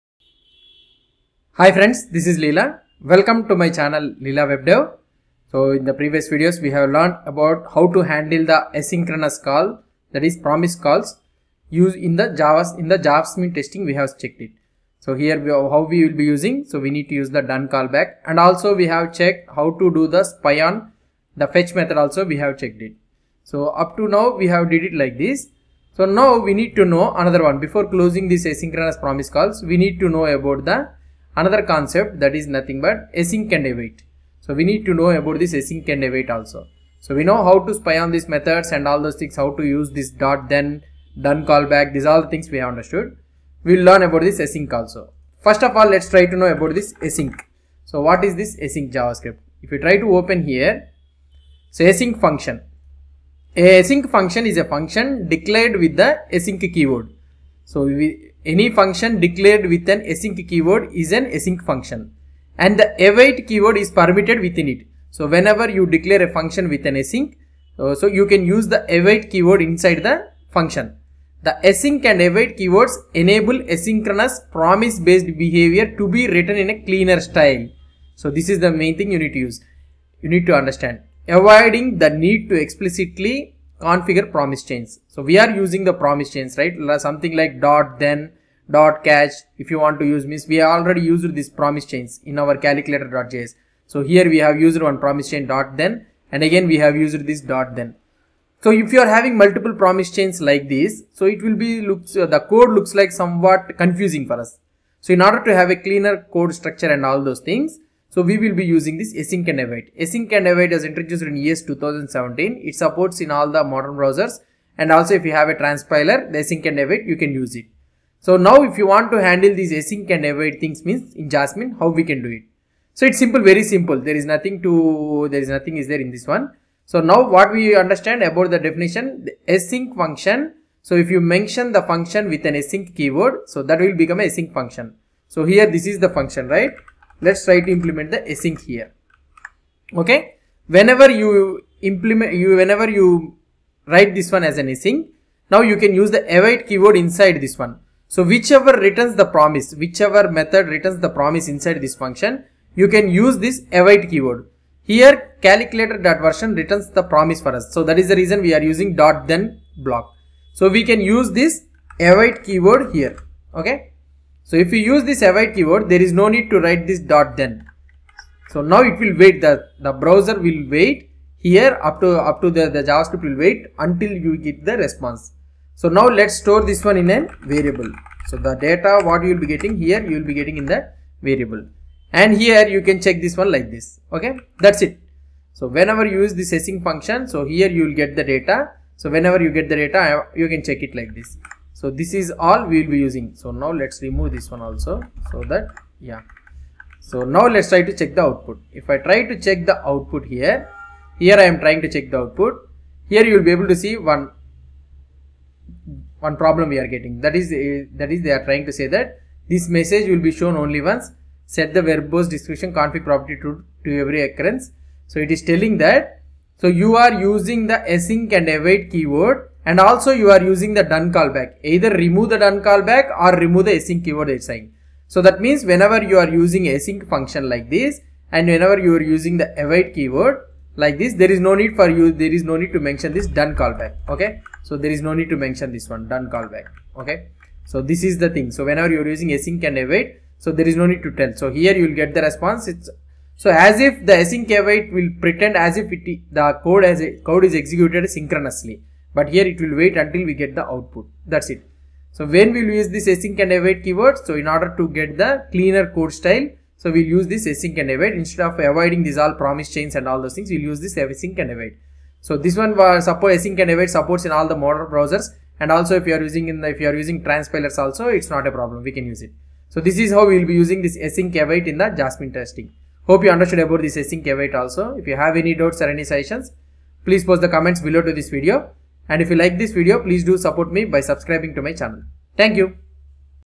58. Testing JavaScript async and await calls - Jasmine Testing
In this video we will see how to test the JavaScript async and await calls - Jasmine Testing.